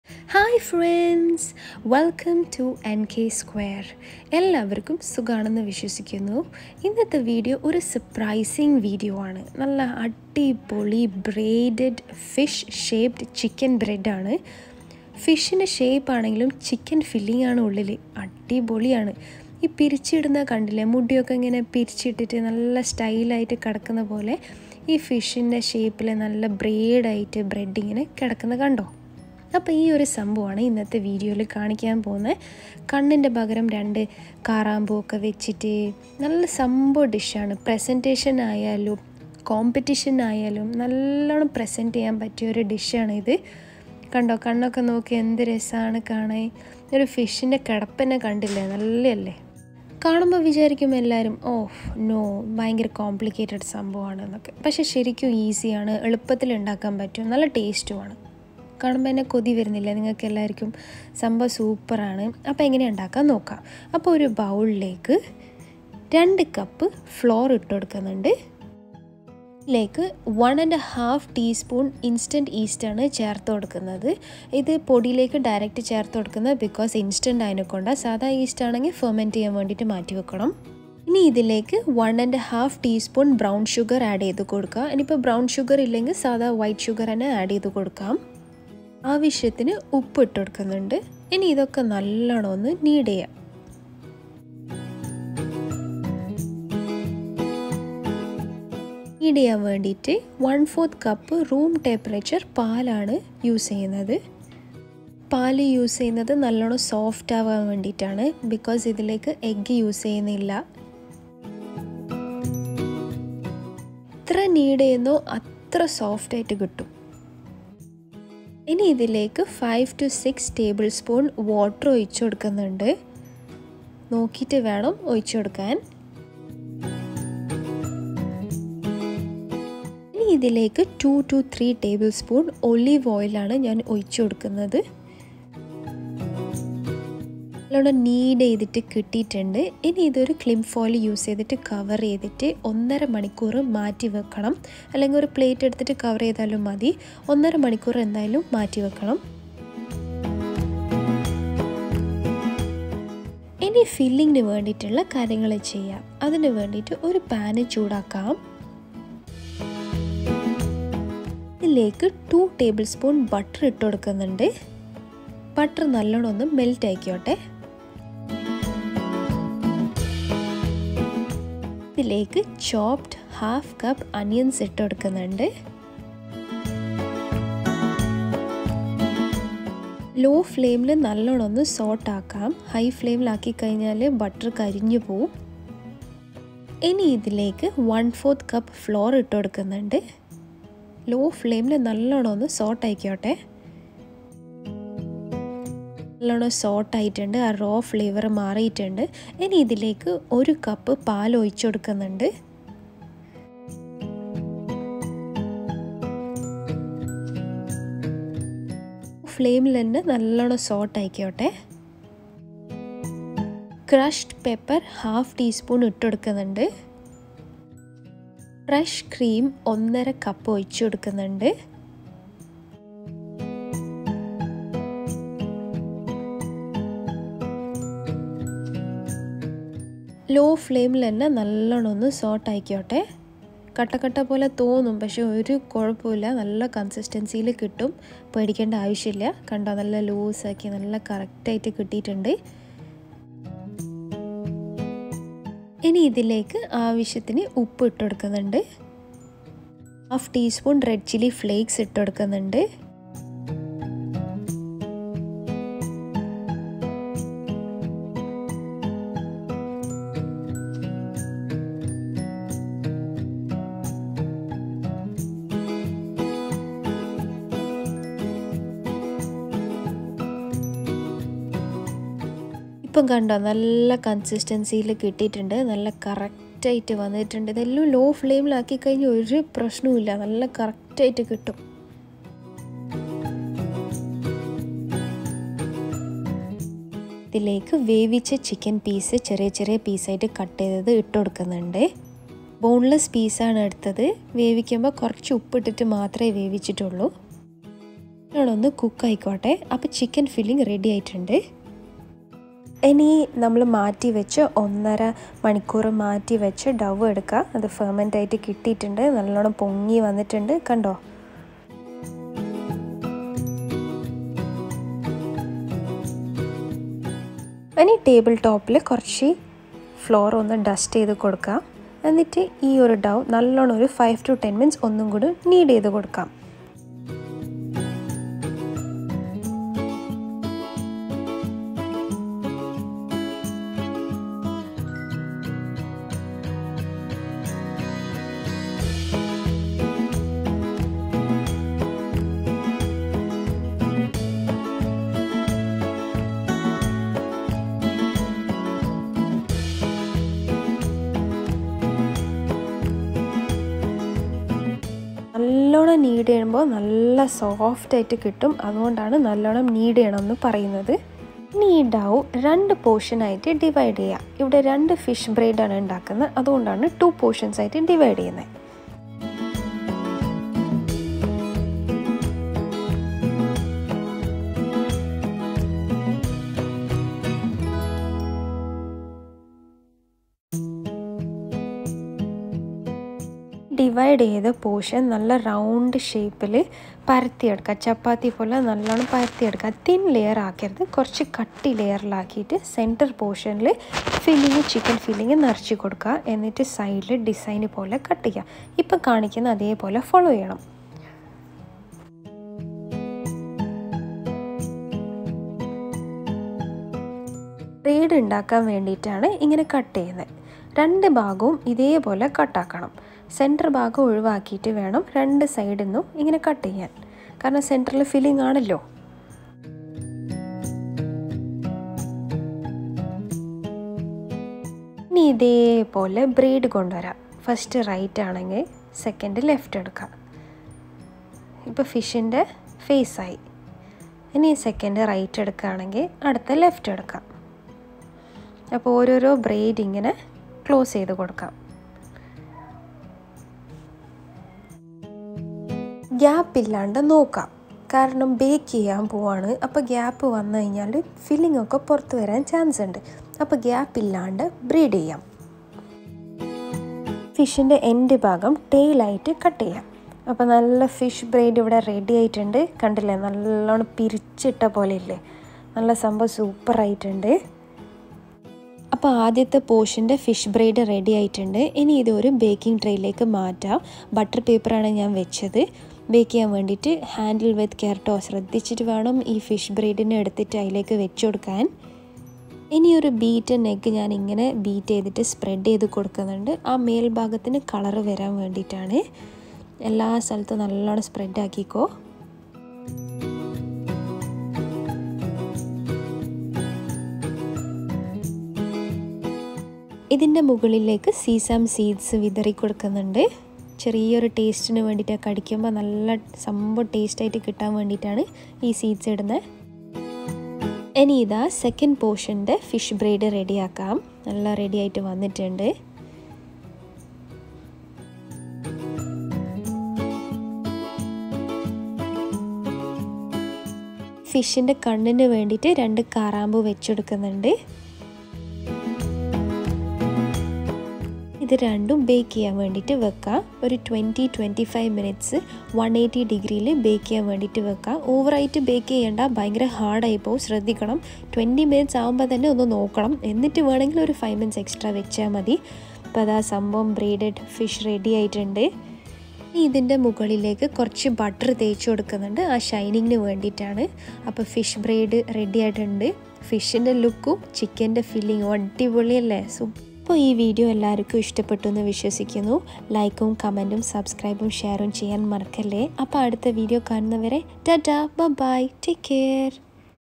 Fish Shaped chicken Braid/Bread | Fish Braid/Bread with chicken filling | NK Square Moments
HI friends. Welcome to NK Square Moments. Here's a surprising video on Fish shaped Chicken Braid/Bread...
Ingredients:
Dough:
-Maida (Flour) 2 cups
-(Yeast) instant 1 & ½ tsp
-(Sugar) 1 & ½ tsp
-(Salt) 1 tsp
-(Milk) room temp ¼ cup
-Oil 5-6 tbs or 2 to 3 tbsp
-(Water) warm 5-6 tbs or as required
Chicken filling:
-Chicken Boiled & shredded 1 & ½ cup
-Dhania (Green coriander) 2 tbs optional
-Fresh cream 1 and 1/2 cup

Assembling:
-Chicken filling
- (Egg) 1
- Kalonji (nigella seeds) as required or seasame seeds

Directions:
Dough:
-In bowl, add all-purpose flour,instant yeast,sugar and salt,mix well.
-Make a well,add milk and gradually add oil and mix it with the help of hands.
-Add warm water (as required), mix and knead well until all ingredients are combined.
-Take dough,punch and knead it again and grease the dough with oil.
-Cover and leave the dough for 1 & 1 ½ hour in a warm place.
-Knead dough again,cover and rest again for 30 minutes.
-Bread dough is ready.
Chicken filling:
-Boil the chicken with salt and black pepper powder, shredded and set aside.
-In wok,add butter,let it melt then add onion and sauté little.
-Add all-purpose flour and sauté until its color changes.
-Remove from heat,add milk,whisk continuously to avoid any lumps formation.
-Bring back on heat,add black pepper powder, salt and mix it.
-Add boiled and shredded chicken and mix well.
-In filling, you can use vegetables or mince (any of your choice).
Assembling:
-Roll out the dough into a rectangular shape with the help of rolling pin.
-Place the filling in the center of the dough.
-Make slits/cuts with the help of knife on both of the longer sides.
-Start bringing the top most slit ends across from each other alternately.
-Keep going, remember to alternate,left to right and right to left.
-Put dough in a baking tray, apply egg-wash,sprinkle sesame seeds then bake in a preheated oven at 180 C for about 20-25 minutes.
-Serve hot.
(Egg-wash means to beat an egg , then brush it all over your bread)

Hope you all enjoyed and liked my video.